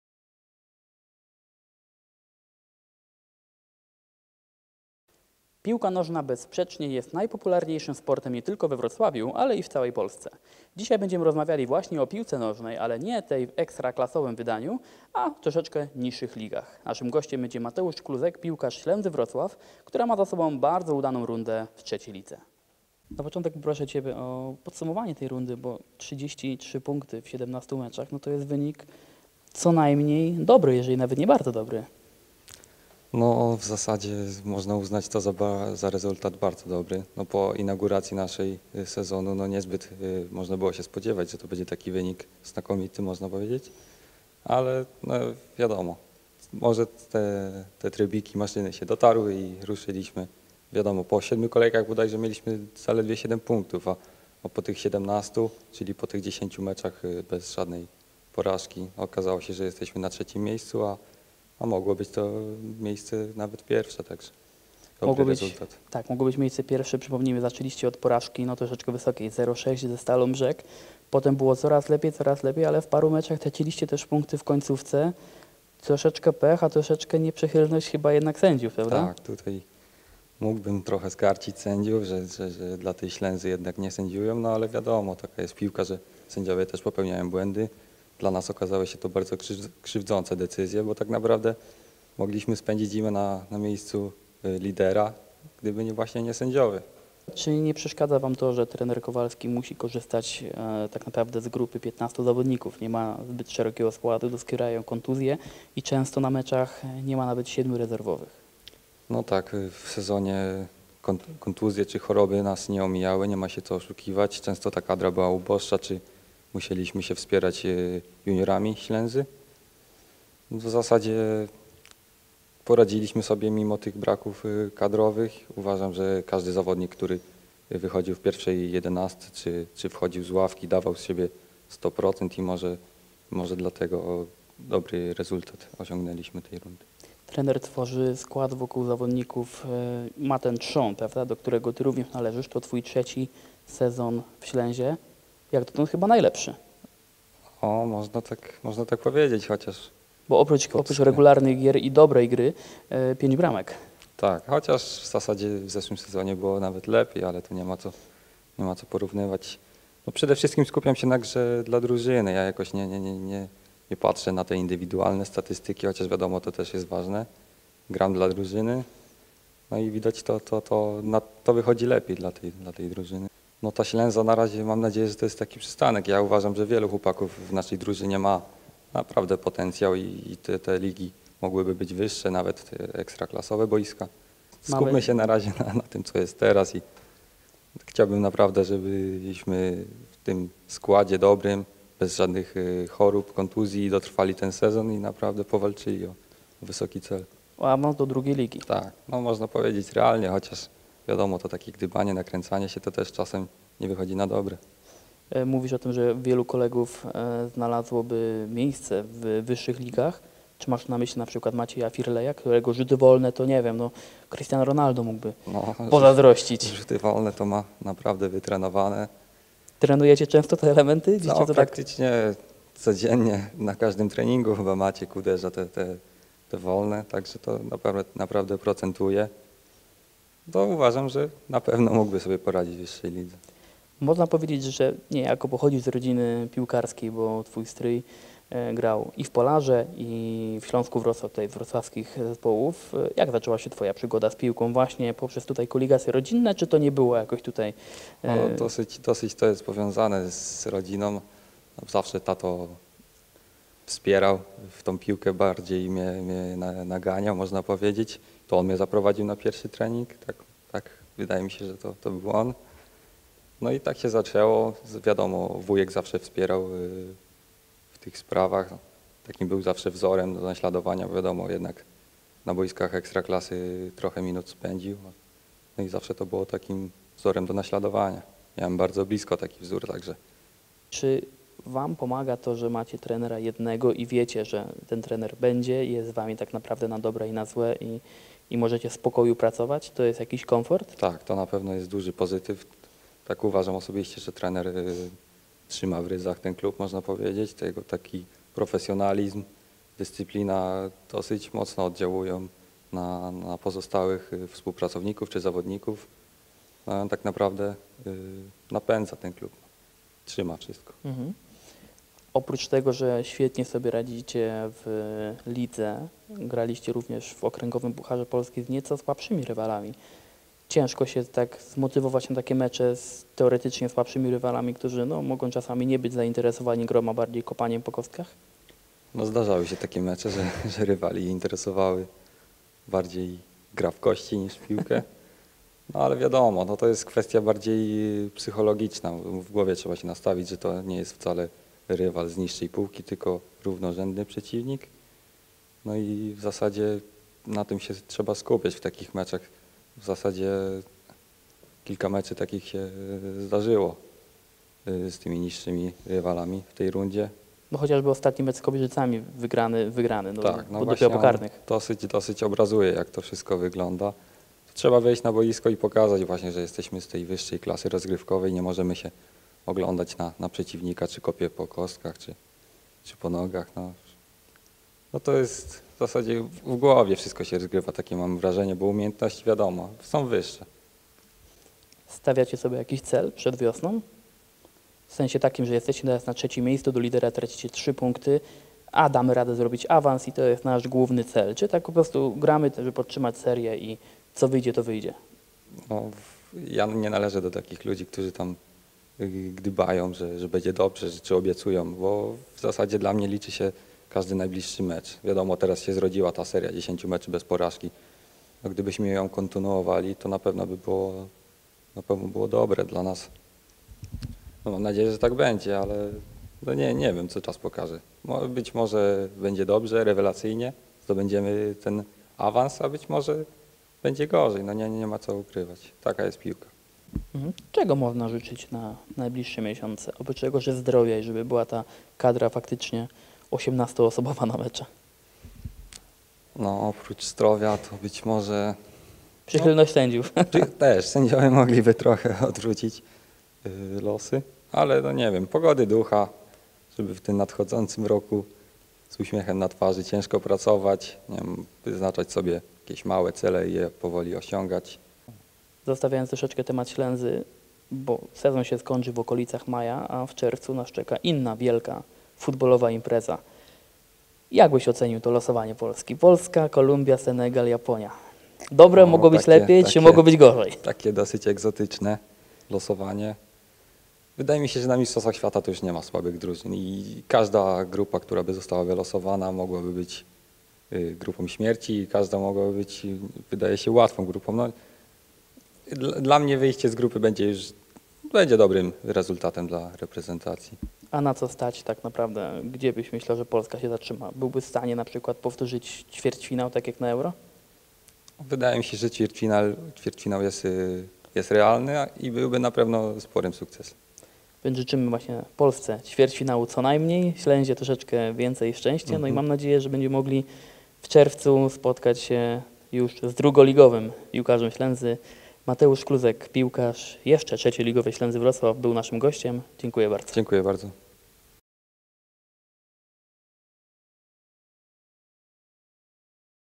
Mateusz Kluzek (Ślęza Wrocław): Trener Kowalski trzyma zespół w ryzach
Mateusz Kluzek - kapitan Ślęzy Wrocław podsumowuje rundę jesienną w wykonaniu 1KS-u, opowiada o celach na wiosnę, a także ocenia losowanie piłkarskiej reprezentacji Polski.